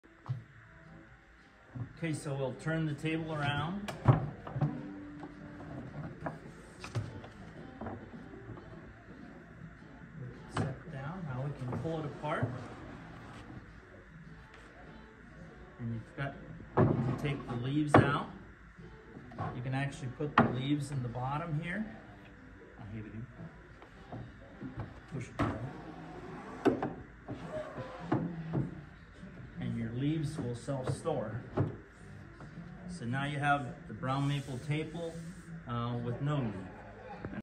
Amish craftsmanship at Fenton Home Furnishings in Michigan
Aden and Ruby demonstrate the construction of an Amish table with self storing leaves.